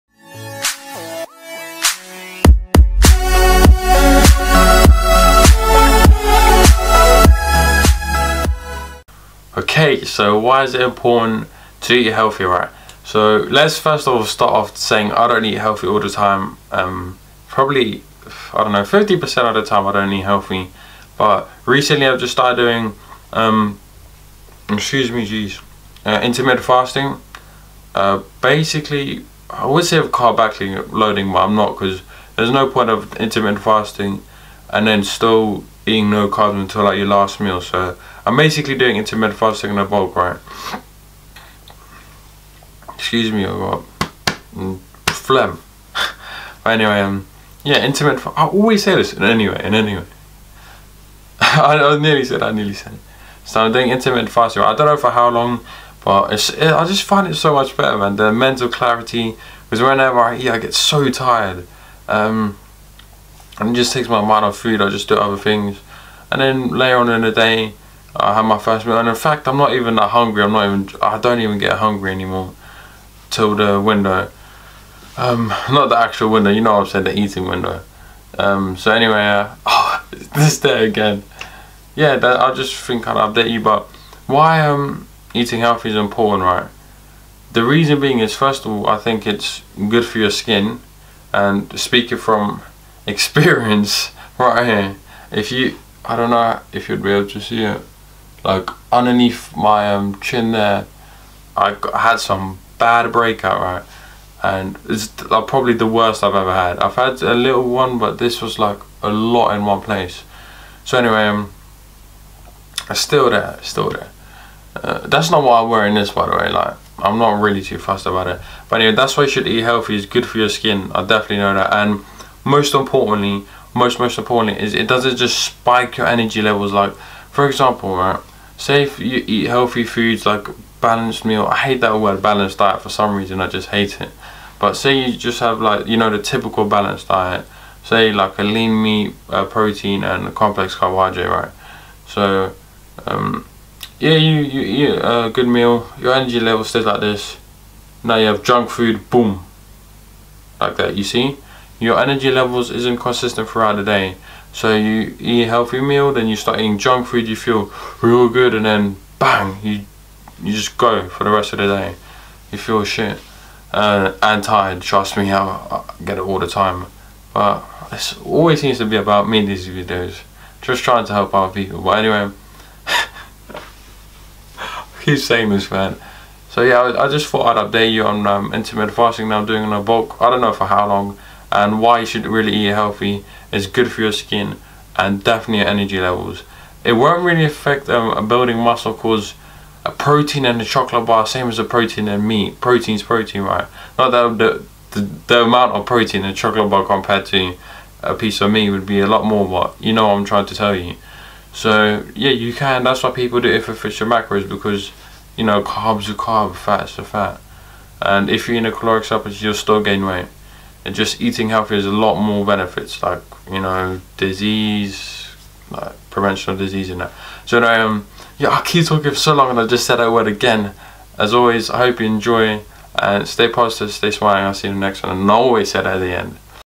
Why Is It Important To Eat Healthy Foods Bodybuilding
If you want to learn How to Lose 30+ Pounds of Stubborn Fat… Without Losing Muscle or “Breaking” Your Metabolism

In this video I will tell you Why Is It Important To Eat Healthy Foods when Bodybuilding.

Although like I always say you don't need to eat any specific foods to lose fat. You just need to be in a caloric deficit!

The number one reason why I think you should eat healthy is because of mental clarity and just normal.health. Your energy levels will be spiked otherwise and your energy levels will be all messed up!

Eating healthy foods when bodybuilding also provides you with better skin as well!

If you have any questions on why is it important to eat healthy foods bodybuilding.